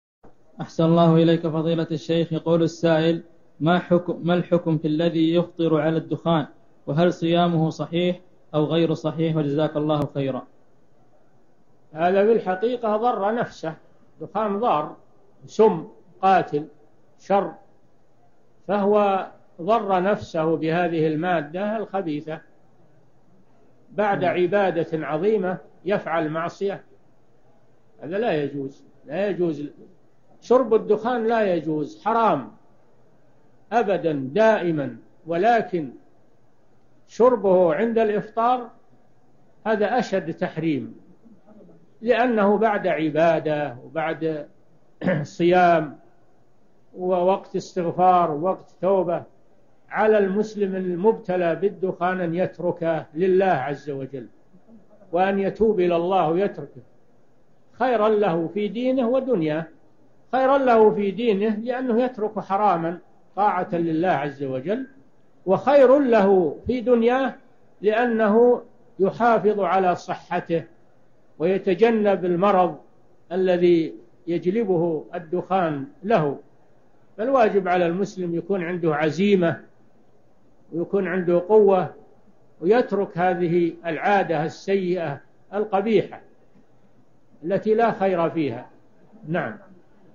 ما حكم الذي يفطر علي سيجارة وهل صيامه مقبول ام لا
ما حكم الافطار علي سيجارة بعد اذان المغرب
حكم التدخين في شهر رمضان
التدخين
السجائر
التدخين في شهر رمضان
هل صيامي صحيح ام لا